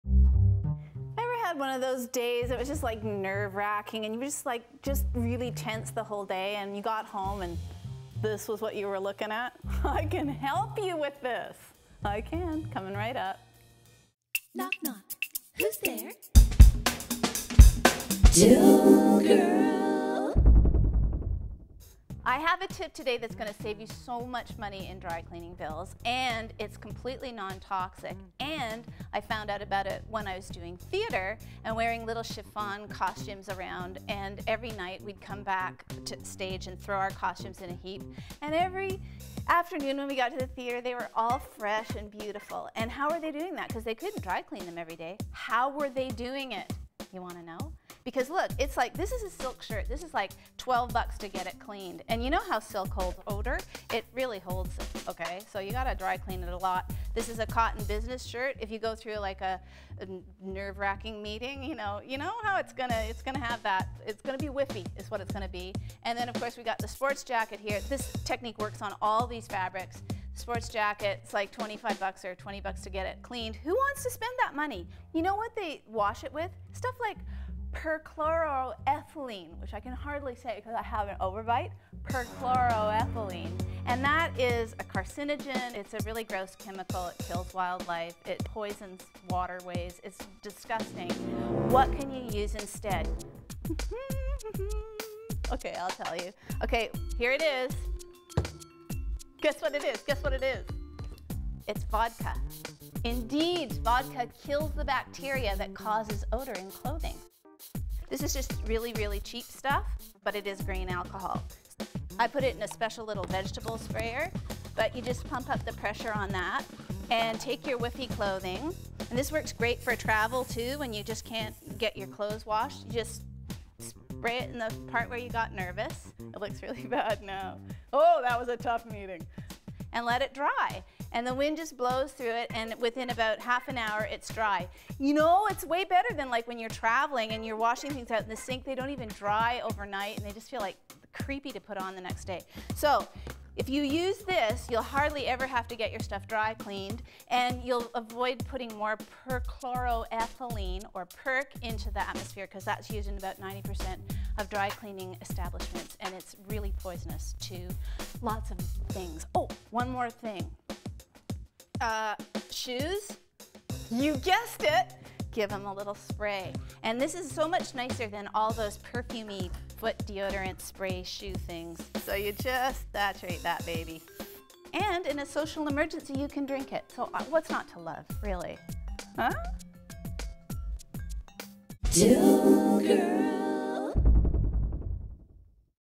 ToolGirl Mag Ruffman - DIY Dry Cleaning
Cut your dry cleaning bills by 90% with this easy non-toxic method